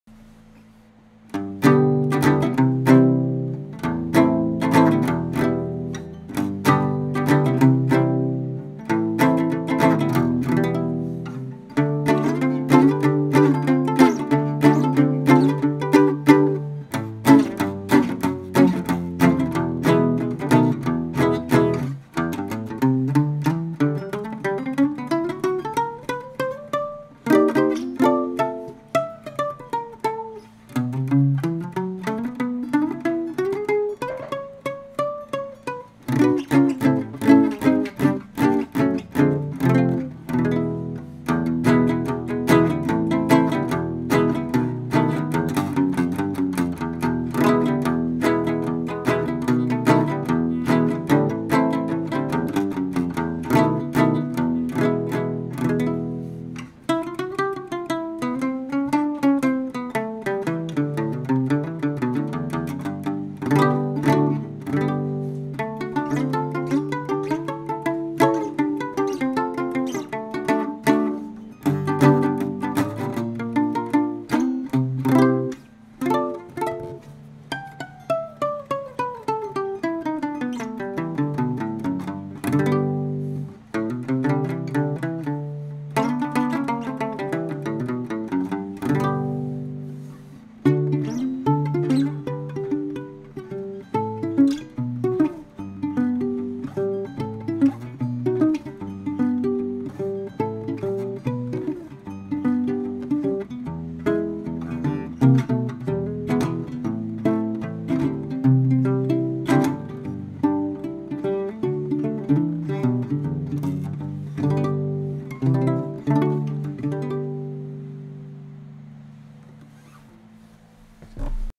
1960s Harmony rubber-bridge parlor guitar (acoustic)
This is the acoustic video — check down my feed for a plugged-in one…

I’ve been working on a batch of rubber bridge parlor guitars — this is one for sale rather than for a customer — a refinished ‘60s Harmony Stella-style, solid birch fella.

Aside from a neck reset, fretwork, and the like I usually do, I’ve also fit it with a single blade-style single coil pickup (sort of Charlie Christian in looks, eh?), wiring harness with Dakaware-style knobs, and cut the original bridge to fit a rubber saddle on it.

Suffice to say, it plays spot-on and has “that sound” — a bouncy, thumpy, sweet, almost muted-marimba sort of voice that’s all the rage these days. A quick roll-off on the tone control gets you a sound more reminiscent of the double-blade humbuckers (read: mellower), but I like the lively thwack the single coils get for this job.